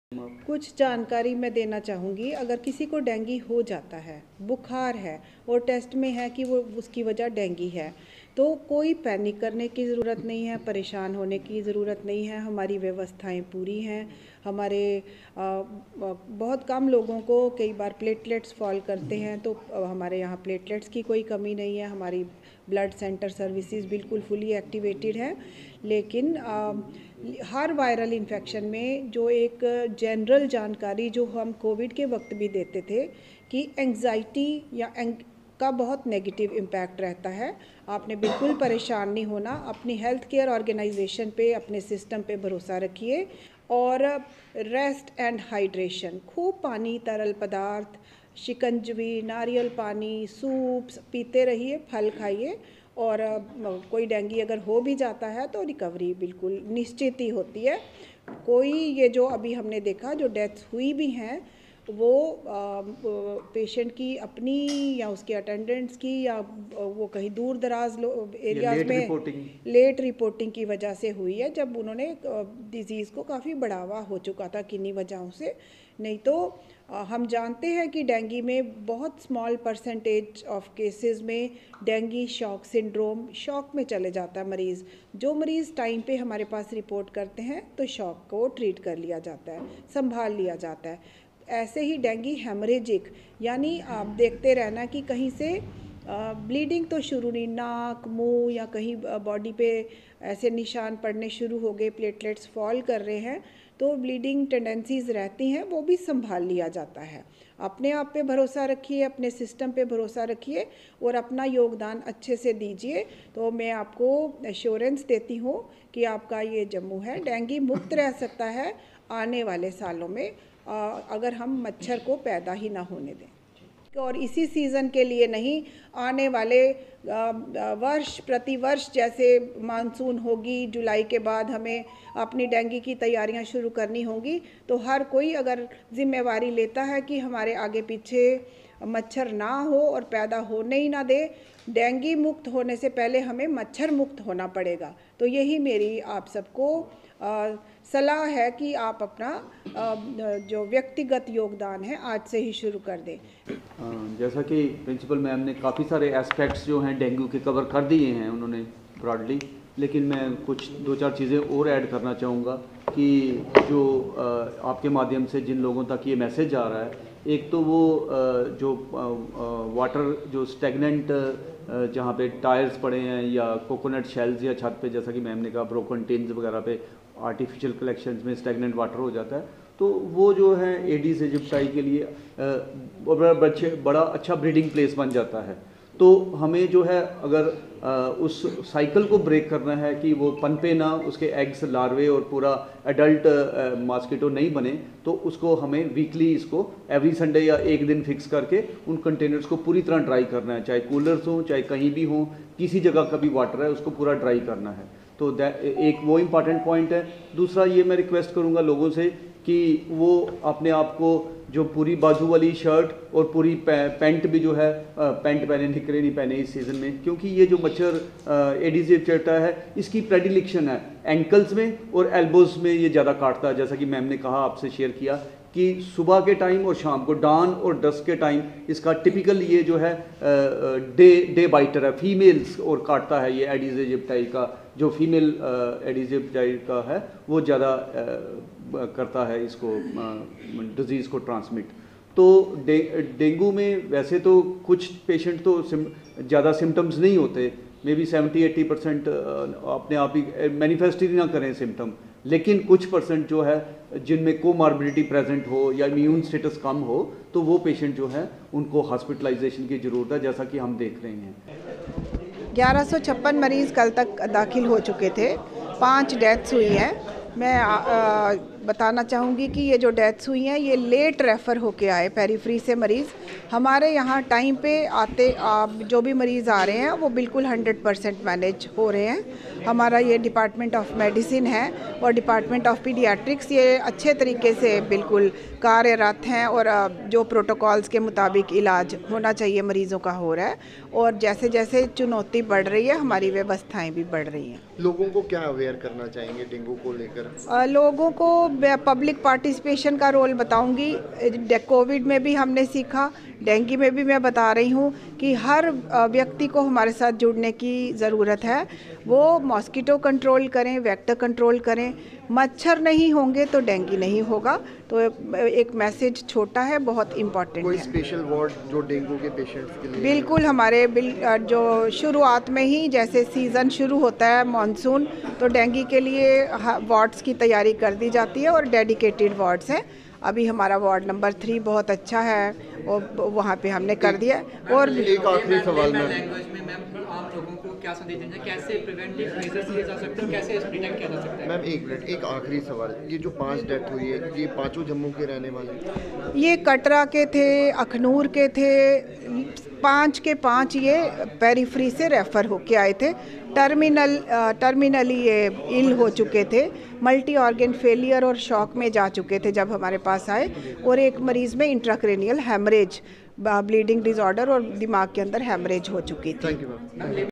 'Remain vigilant to keep dengue at bay, Take precautions, No need to panic': Dr Shashi Sudan
'Remain vigilant to keep dengue at bay, Take precautions, No need to panic': Dr Shashi Sudan, Principal GMC&H